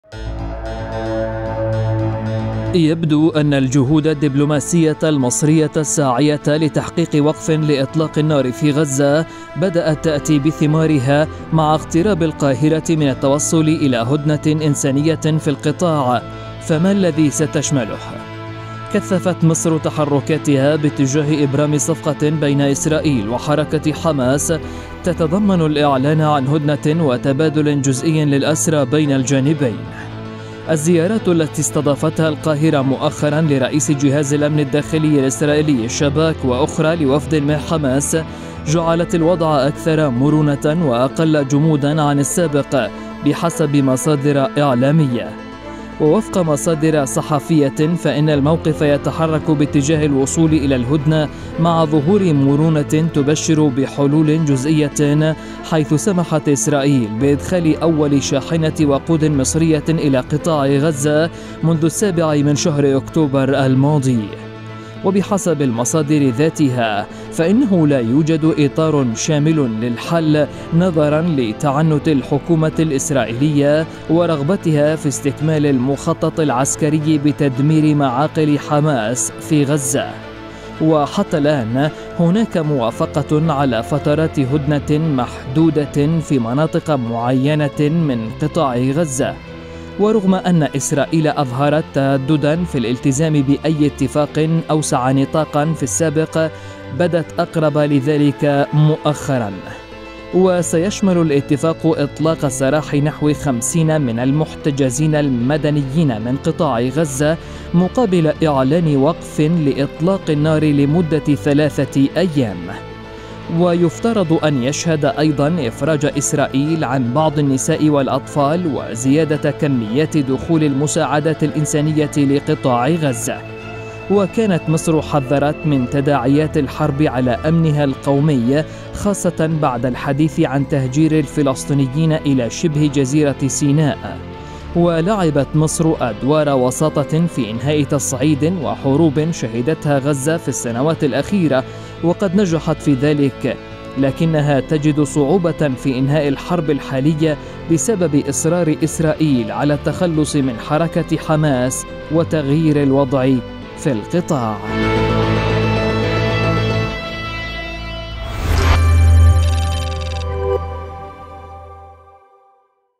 هل تقترب مصر من التوصل لهدنة في غزة؟
تراكمت معلومات من جميع المصادر تؤكد أن تقدماً جدياً حصل في المفاوضات بشأن تبادل أسرى جزئي بين إسرائيل وحماس، فهل تقترب مصر من تحقيق "اختراق" في الصفقة؟